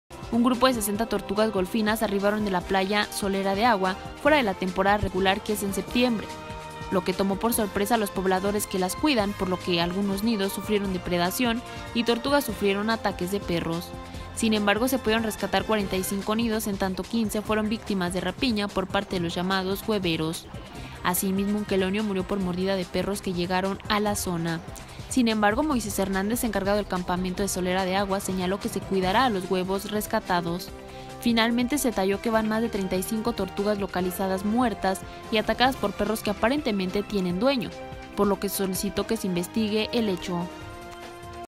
Tortugas realizan desoves fuera de temporada y enfrentan depredación y ataques de perros
Lázaro Cárdenas, Mich.-18 de octubre de 2019 .- Un grupo de 60 tortugas golfinas arribaron de la playa Solera de Agua, fuera de la temporada regular que es en septiembre, lo que tomó por sorpresa a los pobladores que las cuidan, por lo que algunos nidos sufrieron depredación y tortugas sufrieron ataques de perros.

Sin embargo se pudieron rescatar 45 nidos en tanto que 15 fueron víctimas de rapiña por parte de los llamados hueveros.

Asimismo un quelonio murió por mordida de perros que llegaron a la zona.

Sin embargo Moisés Hernández encargado del campamento de Solera de Agua señaló que se cuidará a los huevos rescatados.

Finalmente se detalló que van más de 35 tortugas localizadas muertas y atacadas por perros que aparentemente tienen dueño, por lo que se solicitó se investigue el hecho.